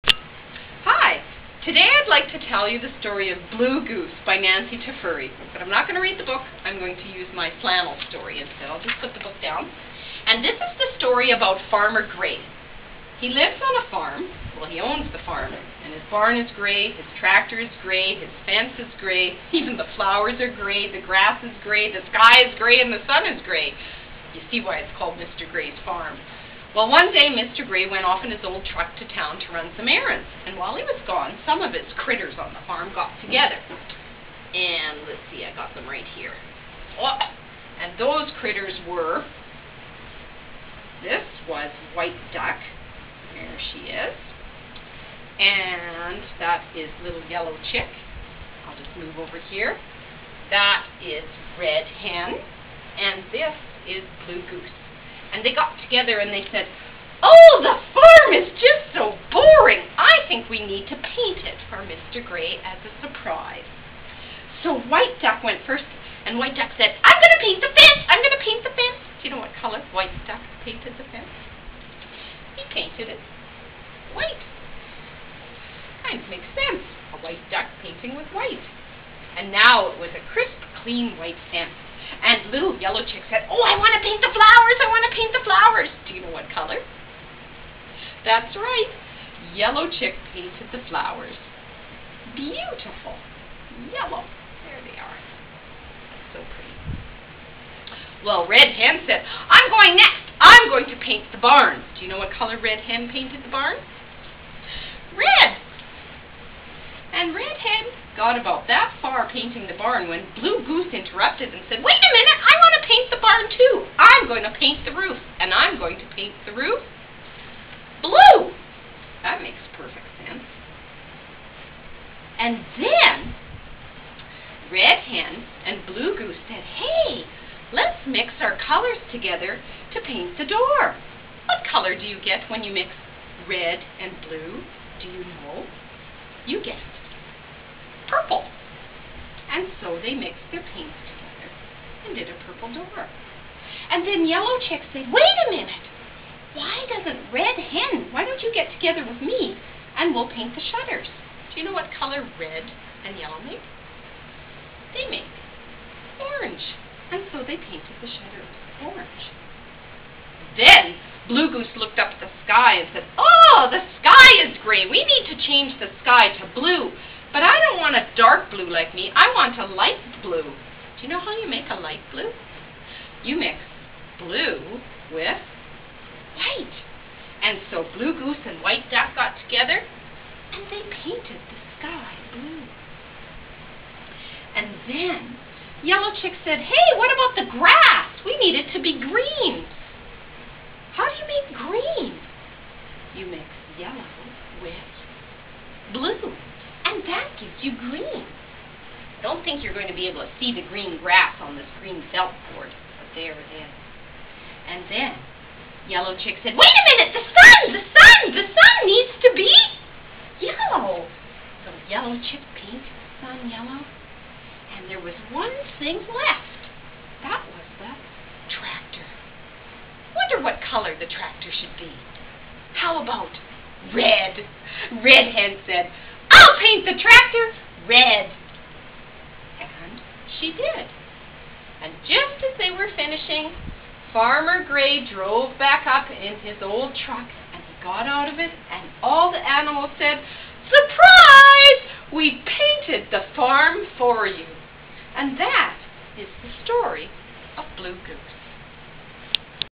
Flannel Story - Blue Goose
Flannel version of Nancy Tafuri's Blue Goose picture book featuring ORL's Youth Services Librarian, Monica Gaucher at the Vernon Branch.